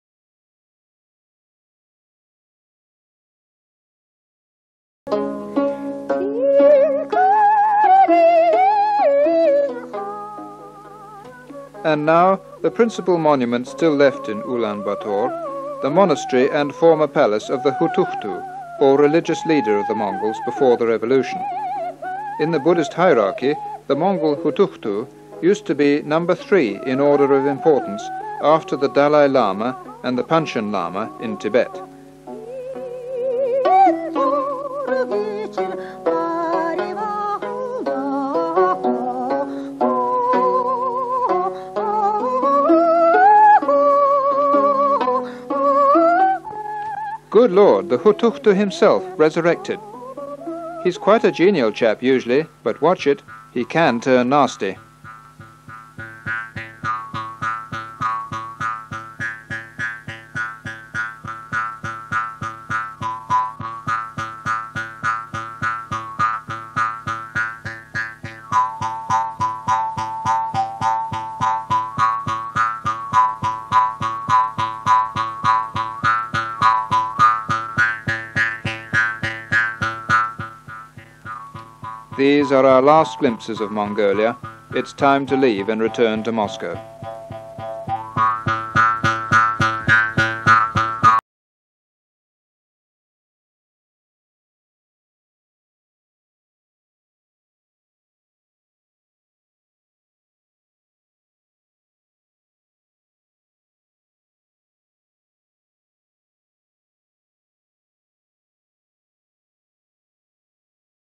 Mongolia 1978 - The Palace of Hutukhtu
Description:
Hutukhtu, in Buddhism, was called the religious-spiritual and political leader and was the head of the Church and the State.
In the film we see footage of a palace and monastery of a Hutukhtu, located in the capital of Mongolia, Ulan Butor.
 We look at the architectural details, doors with illustrations of honorable entities in Mongolia, and in one shot we see the filmmaker disguised as Hutukhtu.
We also watch gardens and corridors to the central pagoda, monks and visitors.
Creator's description:
And now, the principal monument still left in Ulan Bator: the monastery and former palace of the Hutukhtu or religious leader of the Mongols before the Revolution. In the Buddhist hierarchy the Mongol Hutukhtu used to be No. 3 in order of importance after the Dalai Lama and the Panchen Lama in Tibet.
Good Lord! The Hutukhtu himself, resurrected. He’s quite a genial chap usually, but watch it! He can turn nasty.
-Bonar, Andrew Graham